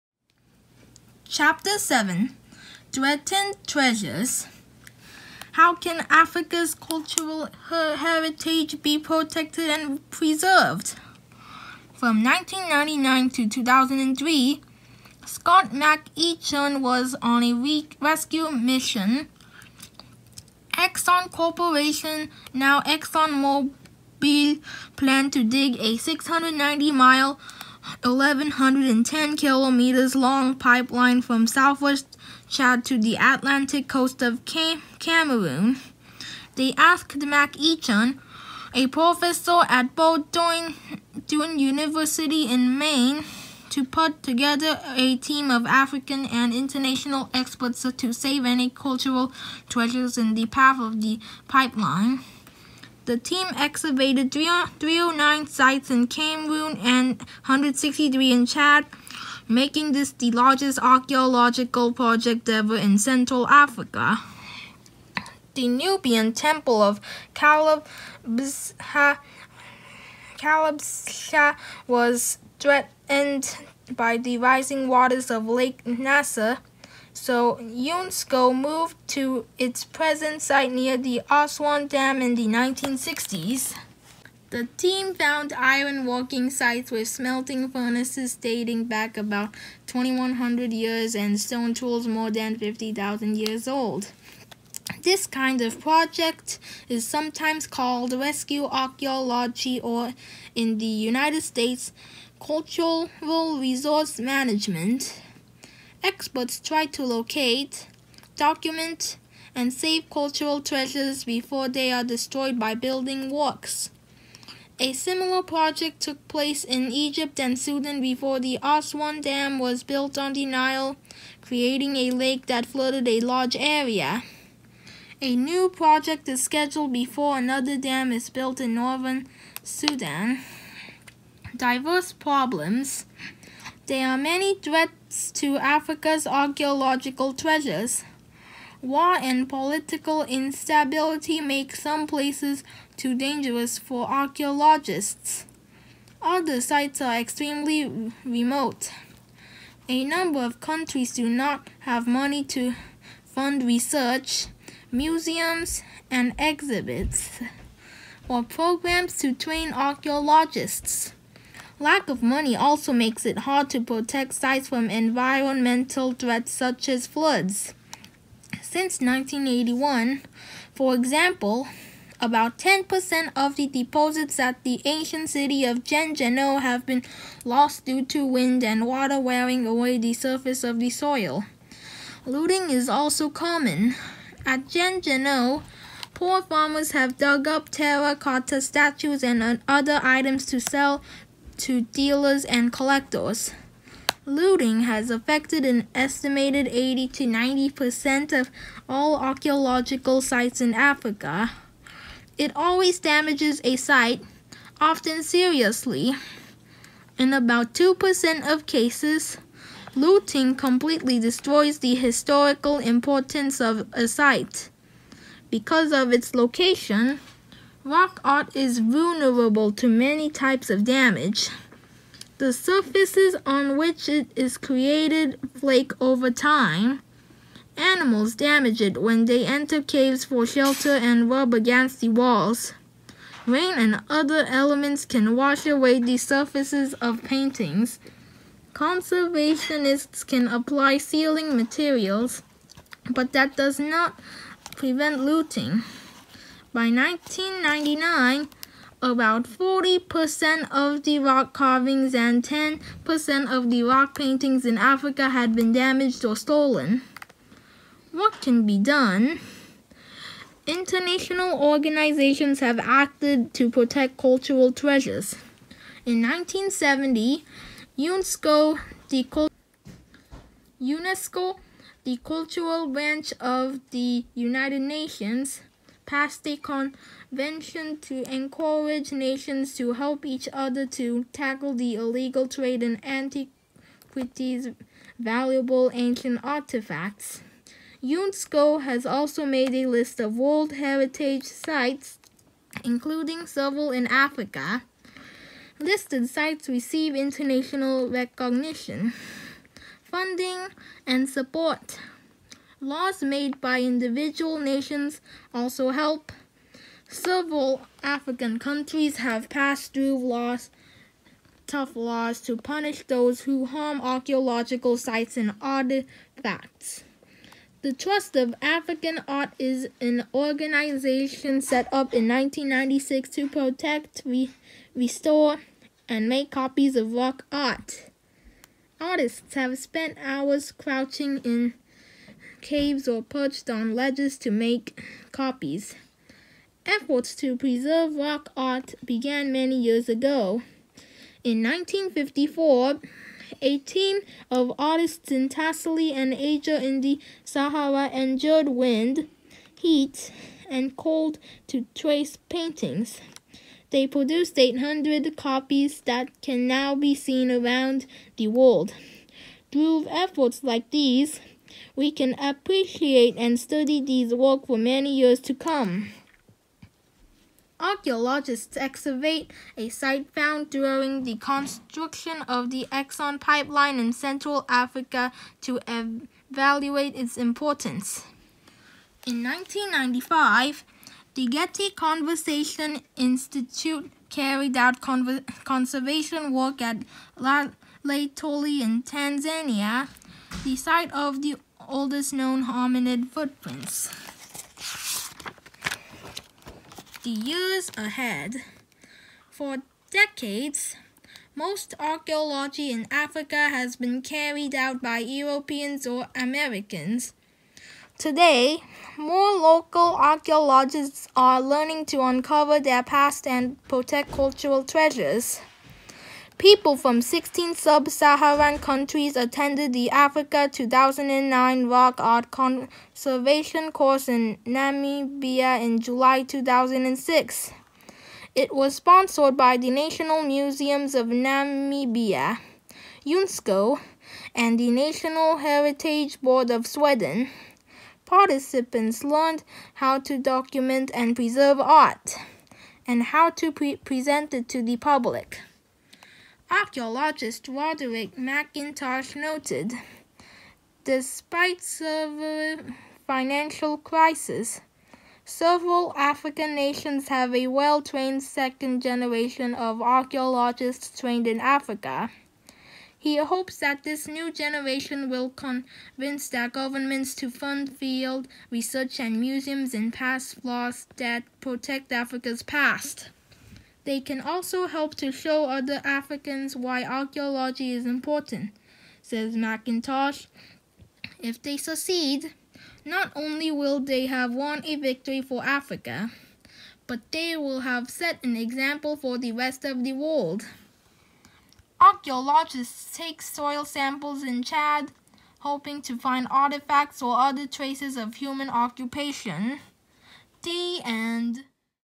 Ancient Africa 9 - #162 영어 책읽기 프로젝트
영어 책읽기 프로젝트 - 캐나다 이민 생활의 가장 중요한 언어에 집중하는 채널입니다.

고대 아프리카 이야기. 잘 알려져 있지 않은 인류의 기원 대륙에 대한 재미있는 이야기들...